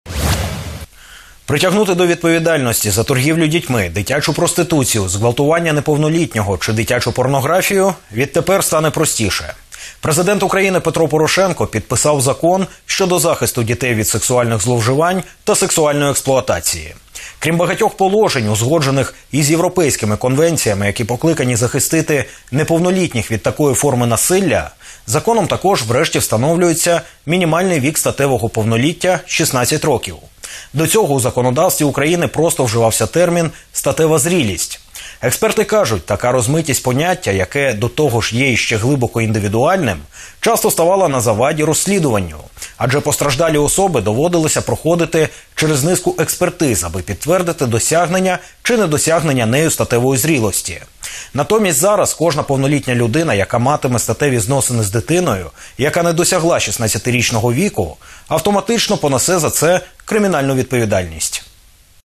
Петро Порошенко підписав закон щодо захисту дітей від сексуальної експлуатації
Притягнути до відповідальності за торгівлю дітьми, дитячу проституцію, зґвалтування неповнолітнього чи дитячу порнографію відтепер стане простіше. Крім багатьох положень, узгоджених із європейськими конвенціями, які покликані захистити неповнолітніх від такої форми насилля, законом також врешті встановлюється мінімальний вік статевого повноліття – 16 років.